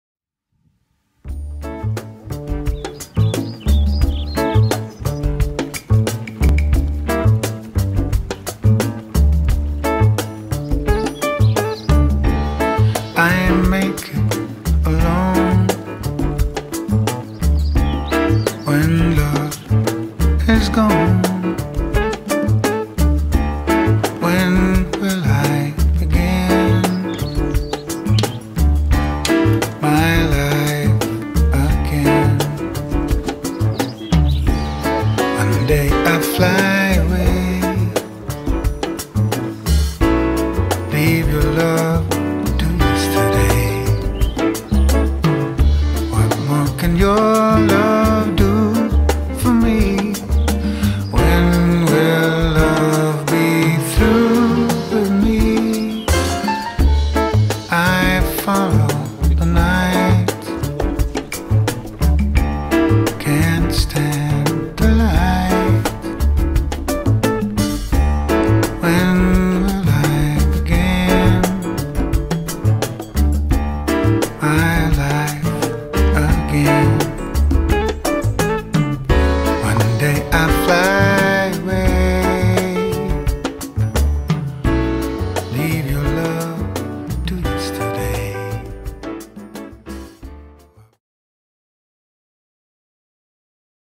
Sand und Salz
Salzwasserpool mit DLW Folienauskleidung in "Sand". Ecktrapeztreppe und Überdachung STAR plus!

Wir wünschen unserem Kunden viel Spaß mit dem neuen Pool und bedanken uns für die Drehgenehmigung!